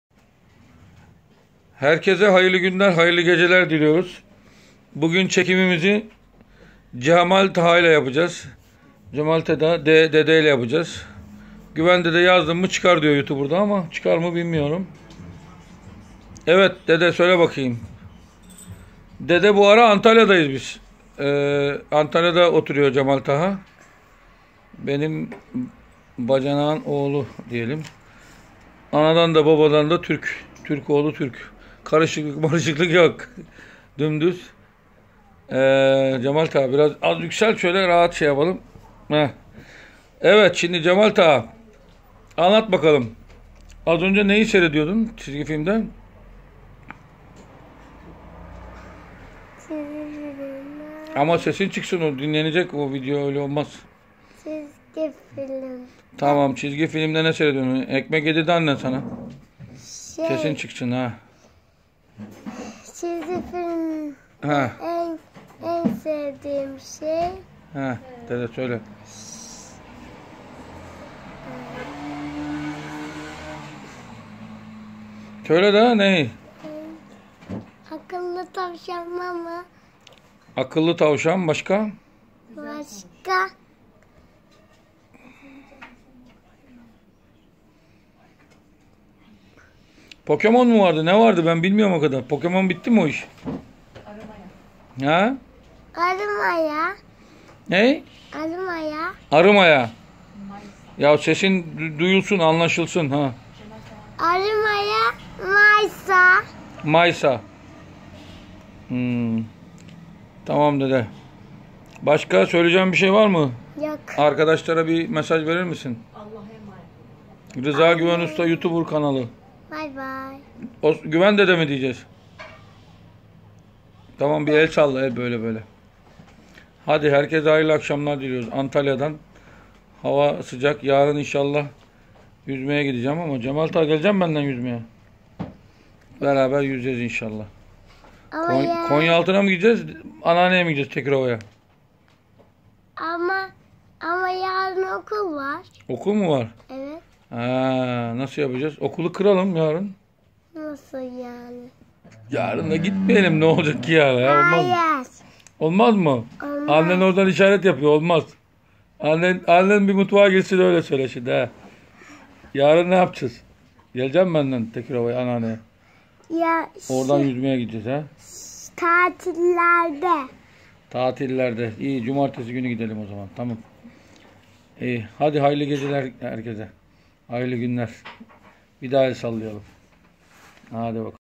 Antalya'da Cemal Taha ile muhabbet ettik.
Güven Dede desinmidi ganal çıkıyor gari.
Cemal Taha izlediği çizgi filimleri anlattı. :)